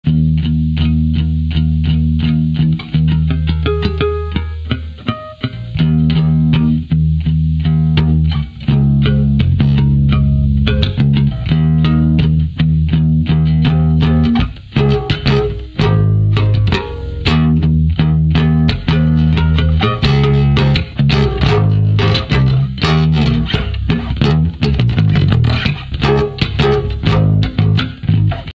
Interval Jam Challenge No. 230430
Challenge where I set the interval timer, it beeps at me and I just have to play something! He's a short of one of them